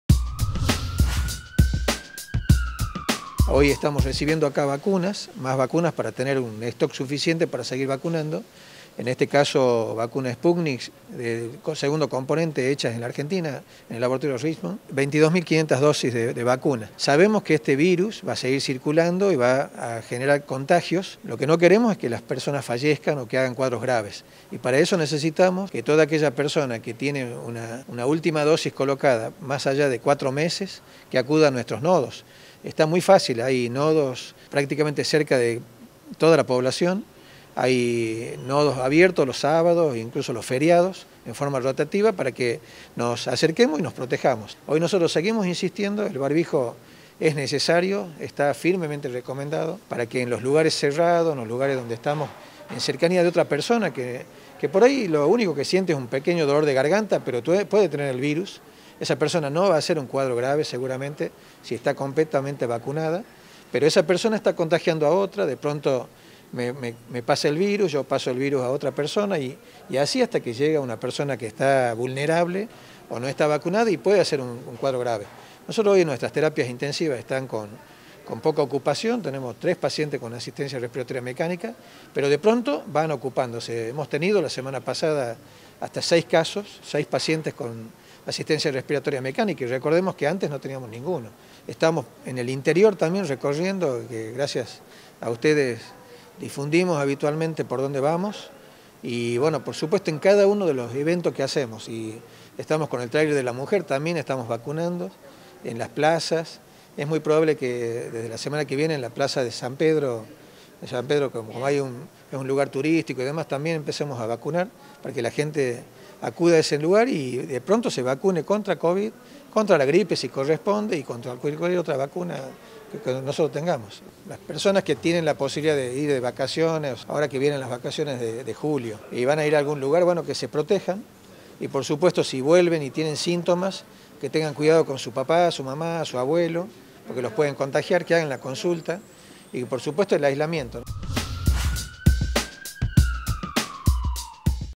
Llegaron 22 500 dosis de SPUTNIK del segundo componente
¨Hoy estamos recepcionando Sputnik del segundo componente fabricadas en nuestro país para ampliar el stock y seguir vacunando como lo venimos haciendo¨, expresó el titular de la cartera sanitaria, doctor Luis Medina Ruiz, desde el depósito central del Siprosa.
¨Este virus va a seguir circulando, los contagios se van a seguir dando, por eso, con este plan de inmunización lo que queremos es que las personas no pasen cuadros graves que impliquen riesgos de vida¨, explicó el ministro.